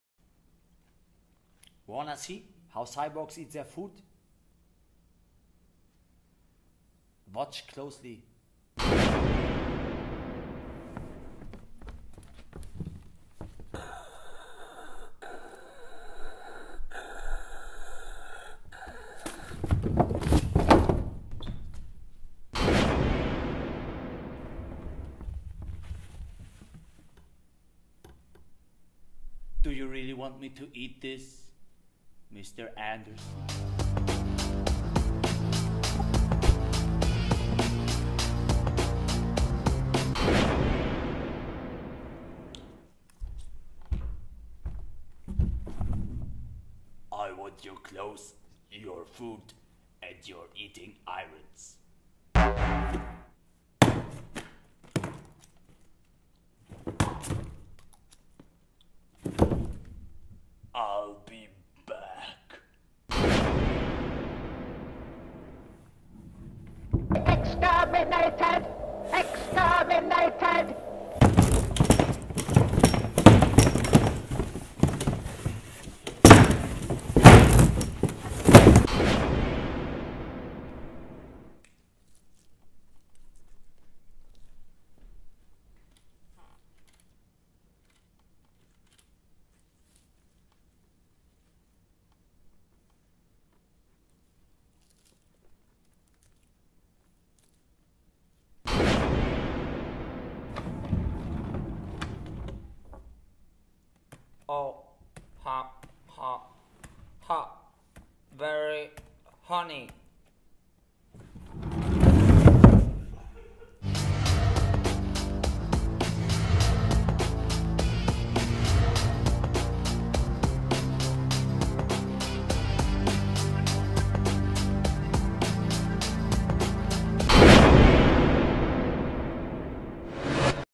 HOW CYBORGS EAT THEIR FOOD - Trailer zum CYBERLEIBER-Festival im Schauspiel Dortmund
In Anlehnung an HOW ANIMALS EAT THEIR FOOD gibt' s vom Schauspiel-Team eine ganz eigene Fassung zum Auftakt des CYBERLEIBER-Festivals 2013.

Theater/Diskurs/Party
6.-9. Juni 2013 im Schauspiel Dortmund